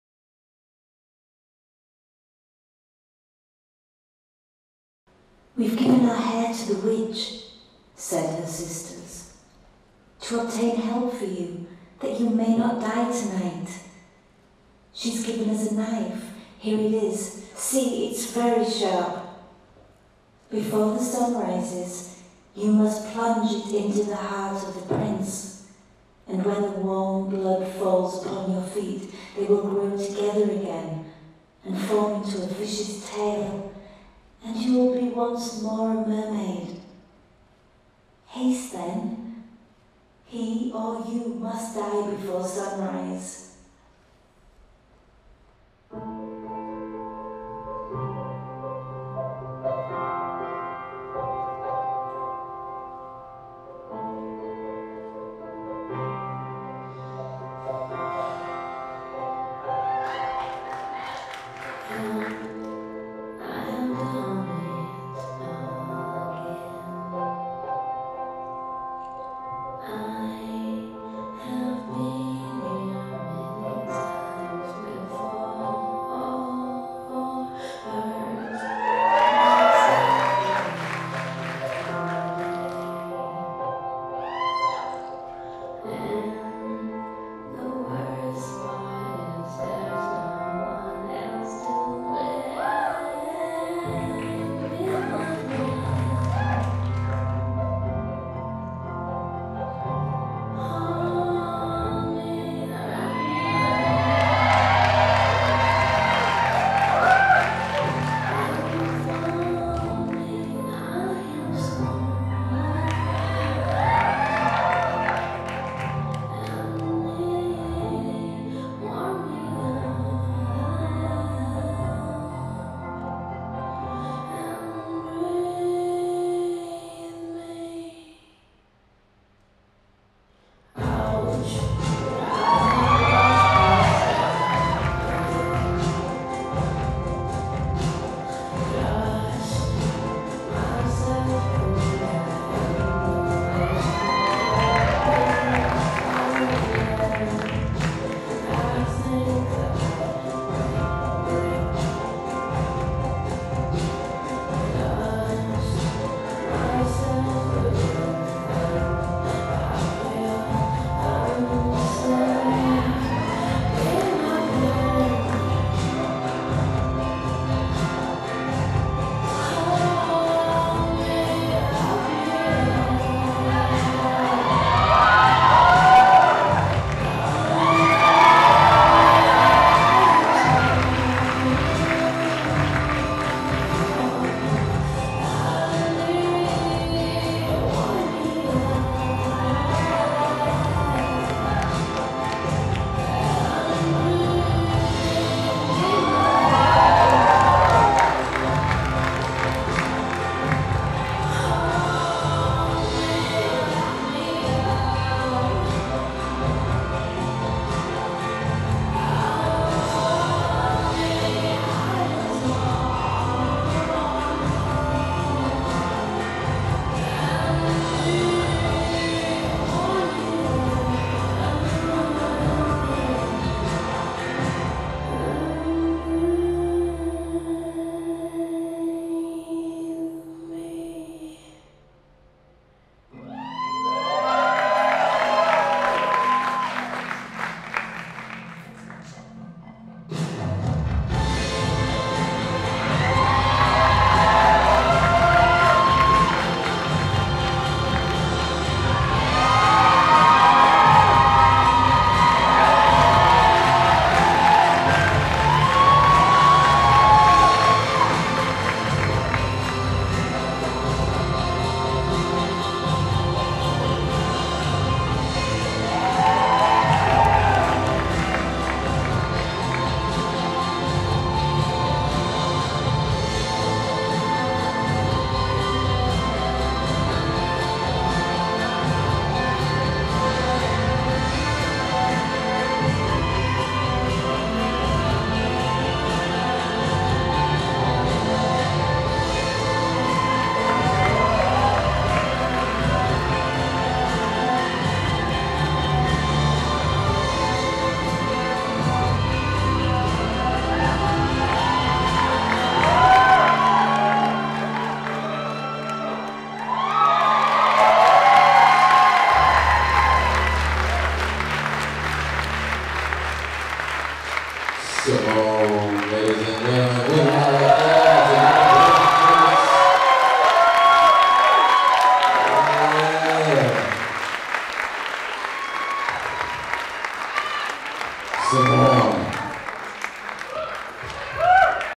Simone - Silks - Aeriform Arts Student Showcase - Once Upon A Time
Simone performing on silks as The Mermaid, during our student showcase, Once Upon A Time.